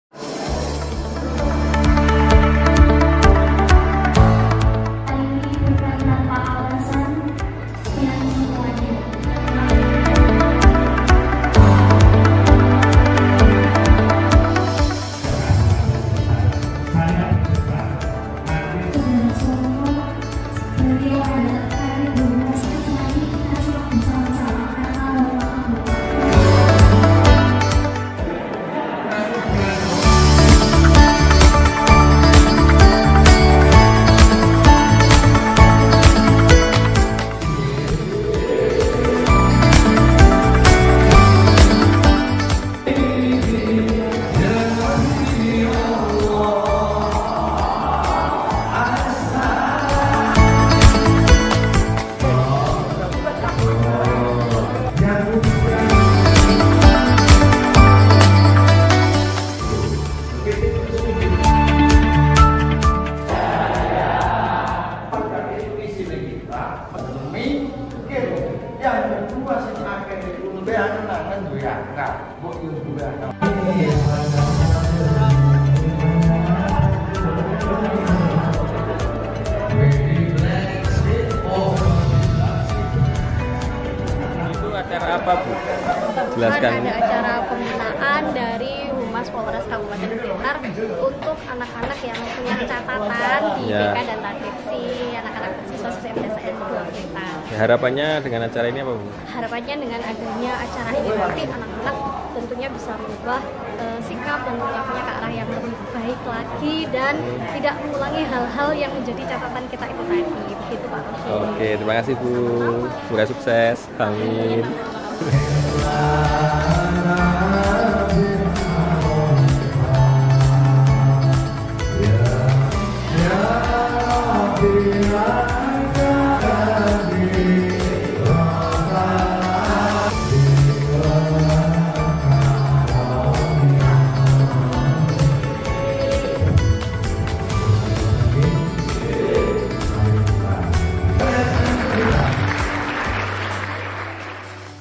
Cegah Kenakalan Remaja Pembinaan Siswa MTsN 2 Blitar : Humas Polres Kab Blitar Aiptu Sudharsono, S.H
Pembinaan Siswa Siswi MTsN 2 Blitar oleh Humas Polres Kabupaten Blitar Aiptu Sudarsono, S H.